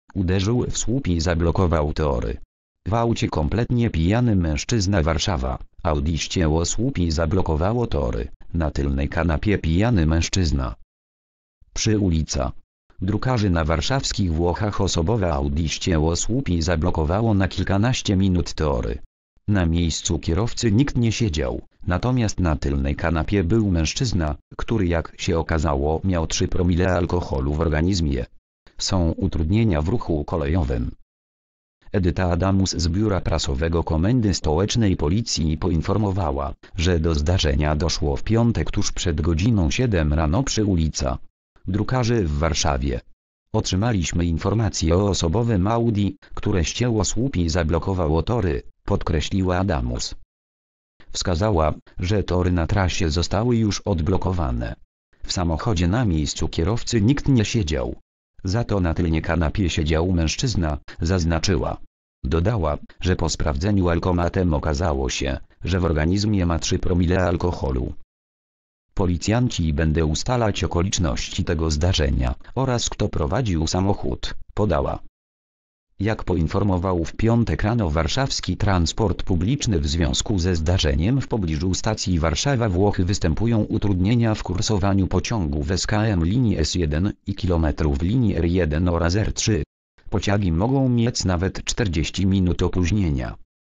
Uderzył w słup i zablokował tory. W aucie kompletnie pijany mężczyzna.
Warszawa: Audi ścięło słup i zablokowało tory; na tylnej kanapie pijany mężczyzna.

  (fot. Shutterstock)